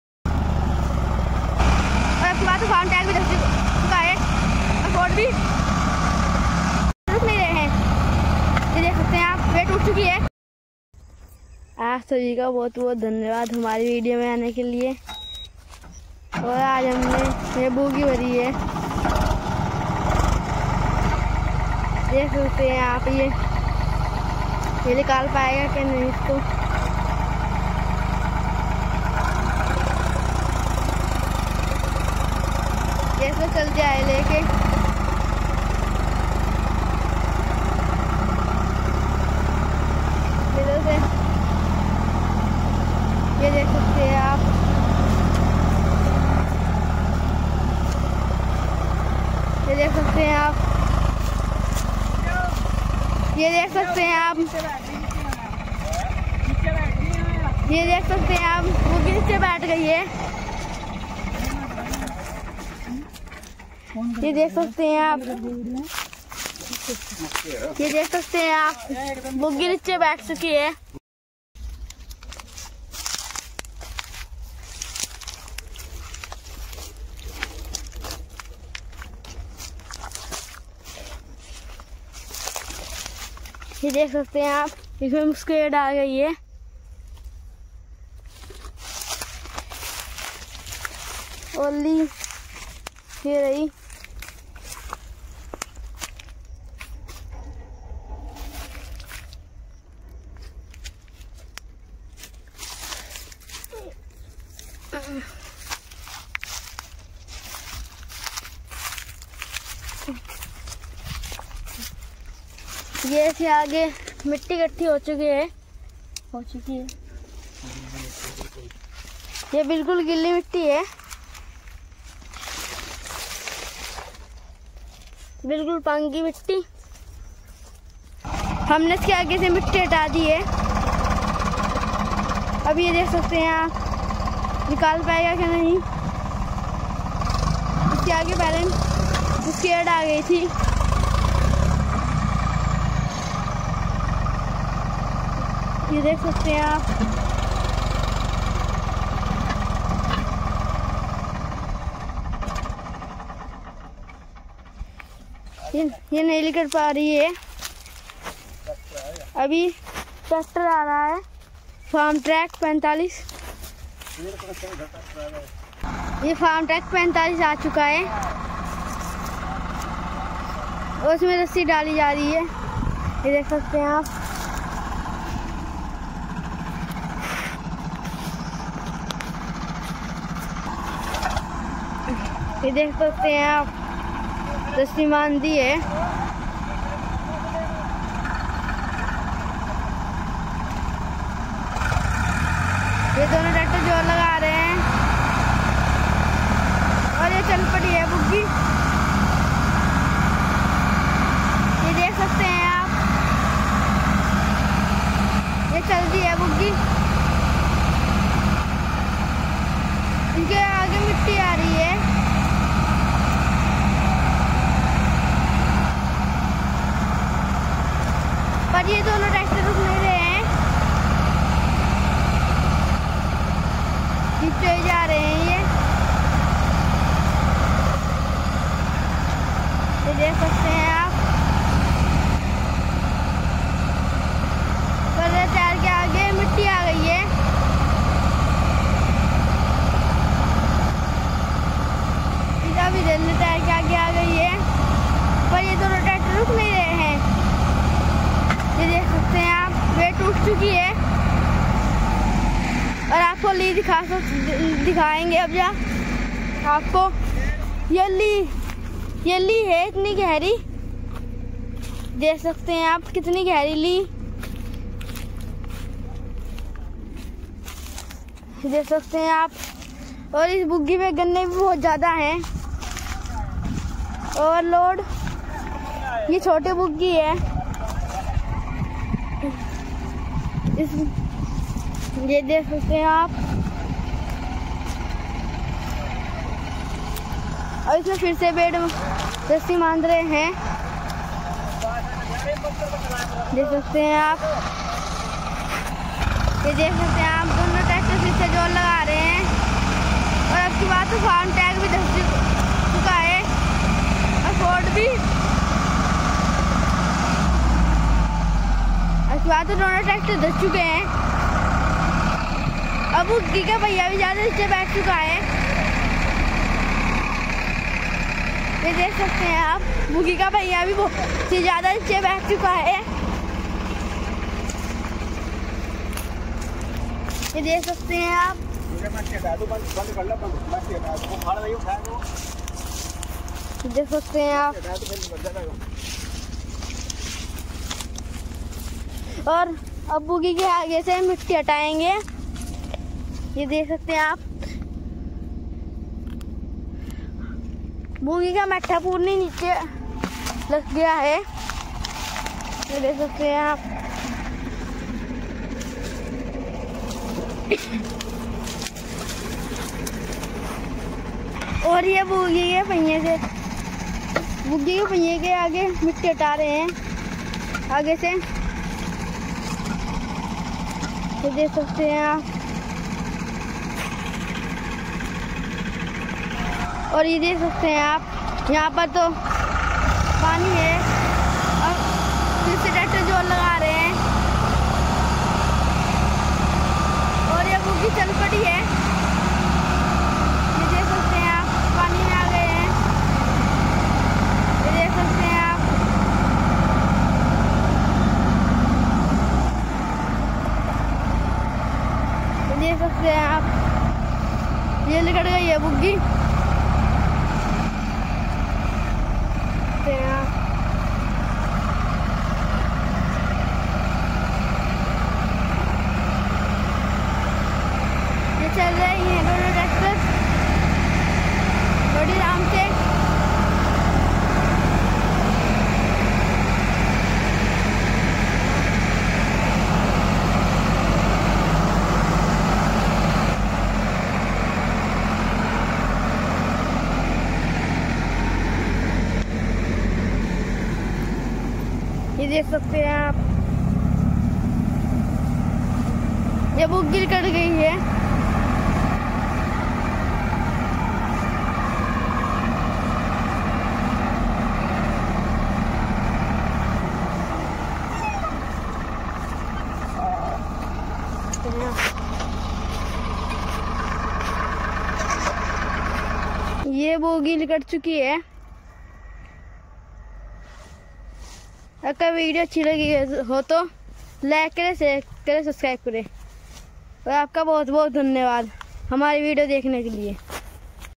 Ford 3600 pulling trolley with sugarcane ll गन्ने की ट्रॉली गीले खेत से कैसे बाहर निकाले...
छोटी सी ट्रॉली दो दो ट्रैक्टर।। Ford 3600 Farmtrac 45...